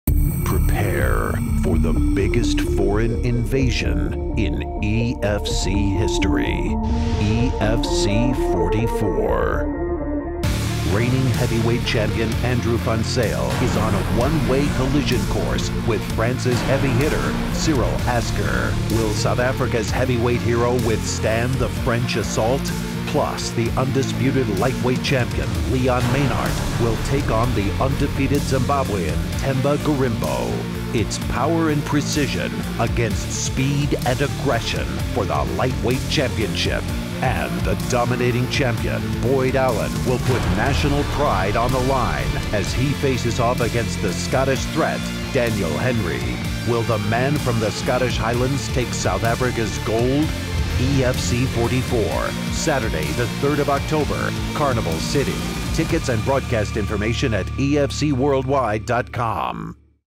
EFC 44: Van Zyl vs. Asker, Mynhardt vs. Gorimbo, Allen vs. Henry
EFC returns to Carnival City on3 October with a triple title fight. Headlined by heavyweights Andrew van Zyl vs. Cyril Asker and the co-main event Leon Mynhardt vs. Themba Gorimbo for the lightweight championship and Boyd Allen vs. Danny Henry for featherweight gold. Full details on EFCworldwide.com